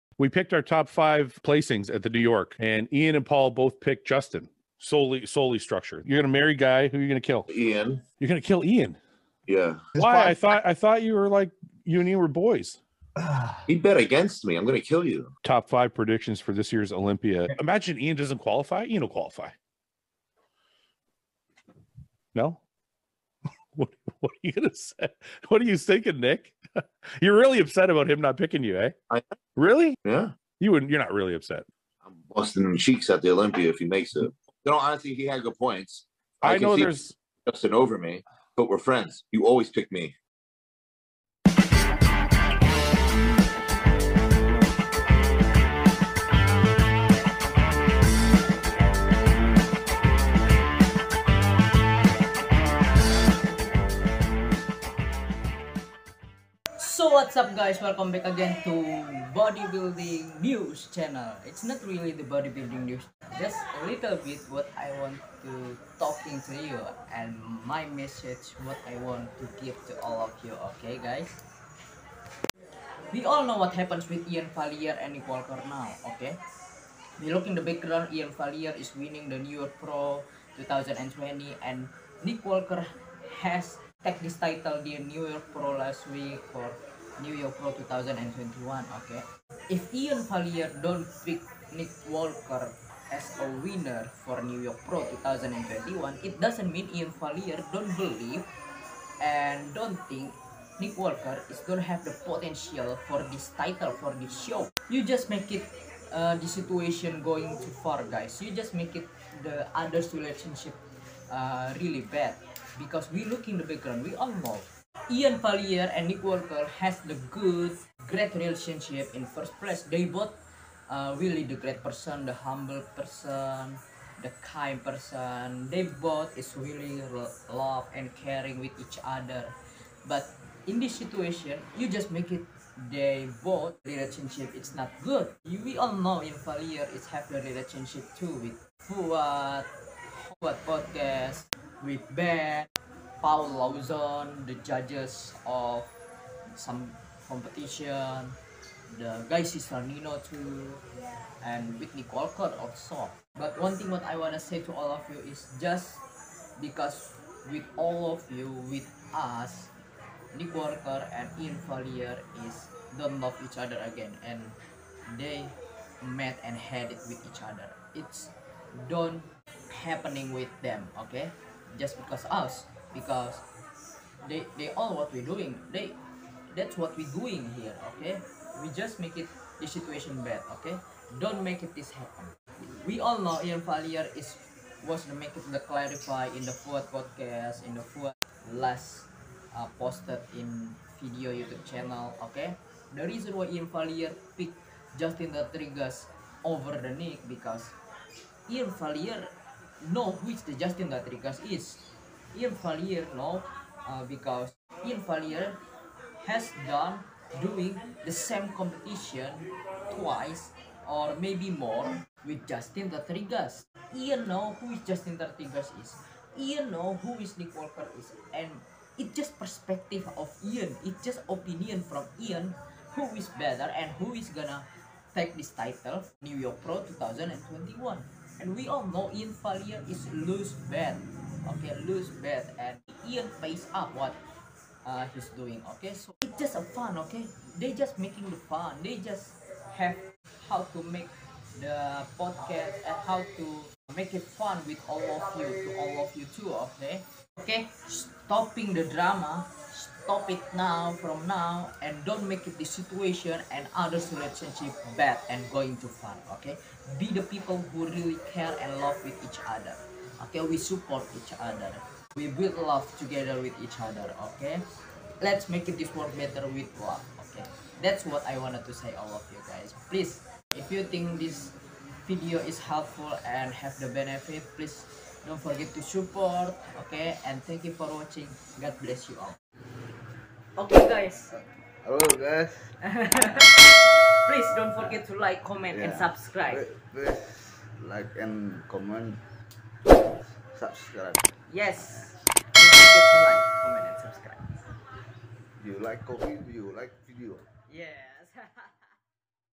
Nick Walker Angry and Mad with Iain Valiere? My Message to All Of You.
This video is wanna Talk and give my Message with People who are really Make the Others Relationship Bad and Going too Far. You Guys Need To Stop this.